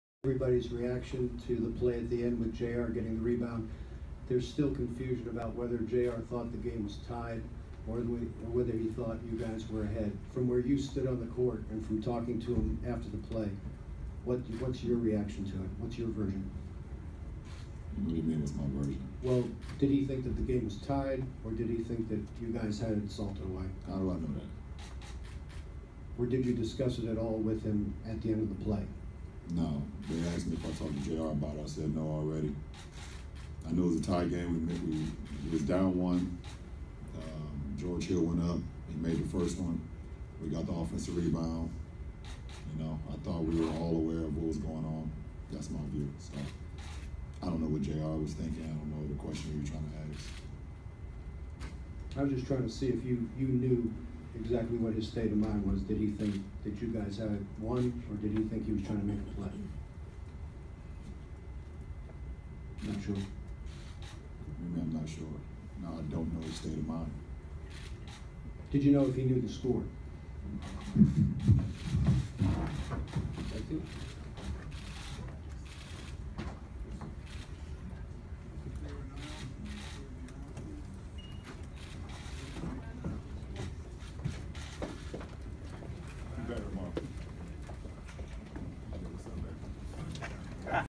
Lebron walks out of Interview after reported keeps asking about Jr Smith!
Lebron walks out of Interview after reported keeps asking about Jr Smith! (via Sports illustrated)